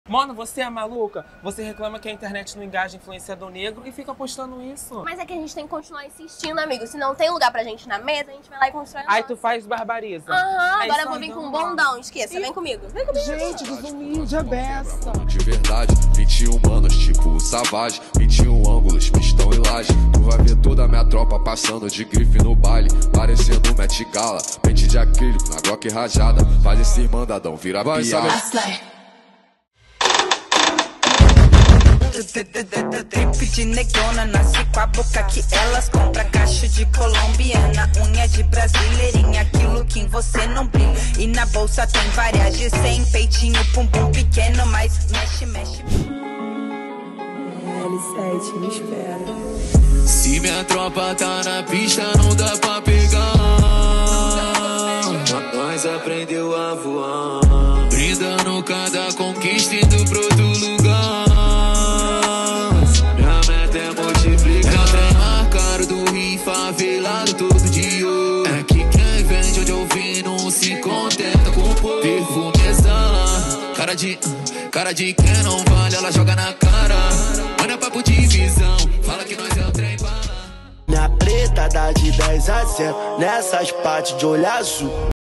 REFS BRASIL & EMPODERAMENTO - EP 3 | Ramana Borba
Quais artistas negros brasileiros te influenciam ?
Quantos artistas negros vocês apoiam e admiram?
Coloquem aqui o @ deles 🫶🏽
Para finalizar esse projeto tão importante.. uma pergunta para vocês:
O quão forte você acha que é ?

Você acharia o dobro se toda a sua história fosse contada!
Tenha orgulho de tudo o que você passou para você ser quem você é. Ninguém tira o que é seu, ninguém pode ser VOCÊ ✨. “ Influência preta” foi meu projeto para o mês da consciência negra, para lembrar o quanto somos incríveis, talentosos e únicos!

Dançando:
Ramana Borba
Lari Carvalho
Bruno Paiva
Giovanna Vasconcelos
Caio Ribeiro
Vini Santos

Participação especial: Xunimm

Coreo 1: Bruno Paiva
Coreo 2: Vitinho Gama
Coreo 3: Caio Ribeiro

Top: Rolling Brand
Hair: Maprina Cabelos

Filmagem: @f3lipegama
Produção : Melissa Borba

00:01 Se inscreve aqui no canal
00:05 REFS BRASIL & EMPODERAMENTO - EP 3 | Ramana Borba

BEIJOS!
Ramana Borba.